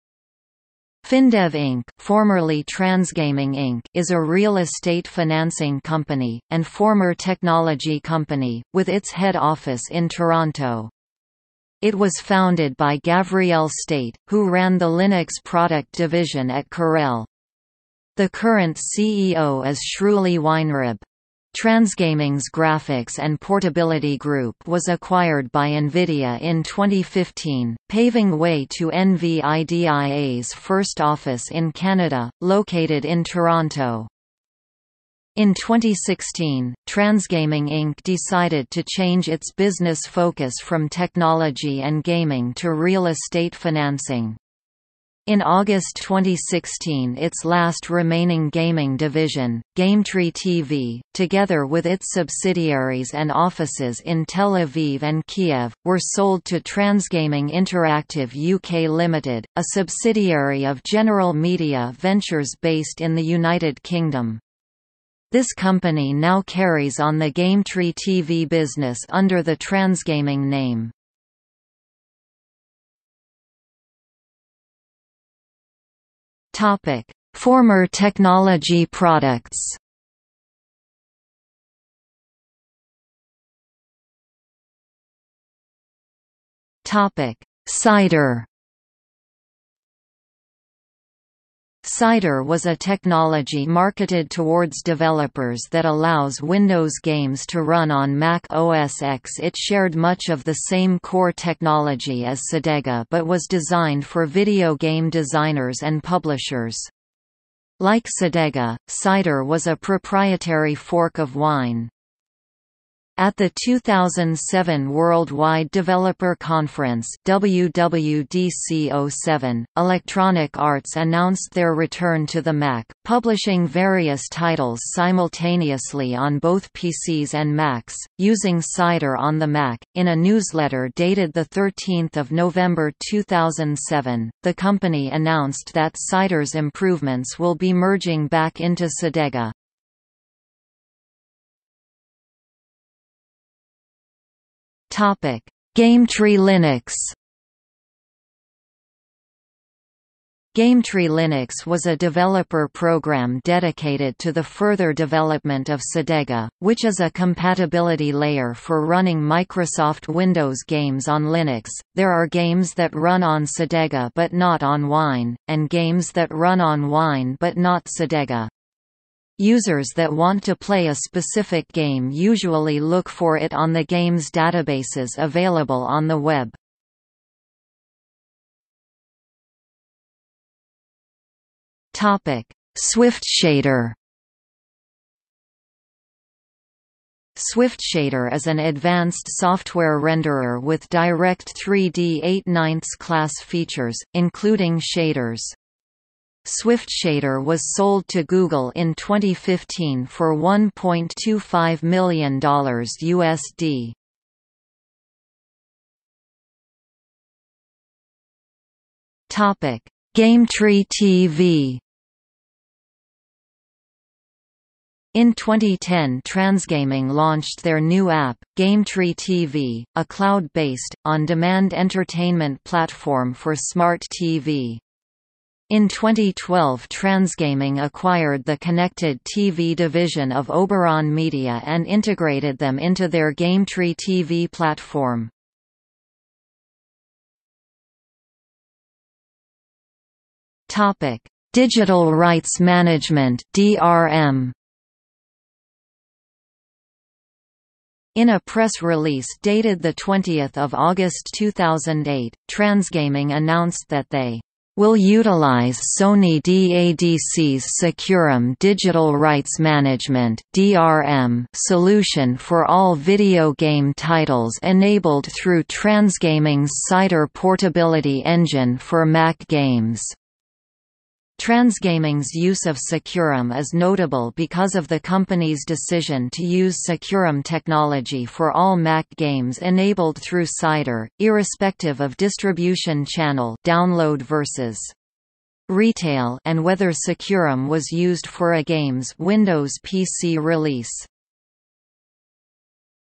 TransGaming Technologies | Wikipedia audio article
00:01:13 1 Former technology products
00:01:23 1.1 Cider
00:02:21 1.2 GameTree Linux
00:02:58 1.3 SwiftShader
00:03:26 1.4 GameTree TV
00:03:57 1.5 Digital rights management (DRM)

- Socrates

SUMMARY
Findev Inc. (formerly TransGaming Inc.) is a real estate financing company, and former technology company, with its head office in Toronto. It was founded by Gavriel State, who ran the Linux product division at Corel. The current CEO is Sruli Weinreb. TransGaming's Graphics and Portability Group was acquired by NVIDIA in 2015, paving way to NVIDIA's first office in Canada, located in Toronto.
In 2016, TransGaming Inc. decided to change its business focus from technology and gaming to real estate financing. In August 2016 its last remaining gaming division, GameTree TV, together with its subsidiaries and offices in Tel Aviv and Kiev, were sold to TransGaming Interactive UK Limited, a subsidiary of General Media Ventures based in the United Kingdom. This company now carries on the GameTree TV business under the TransGaming name.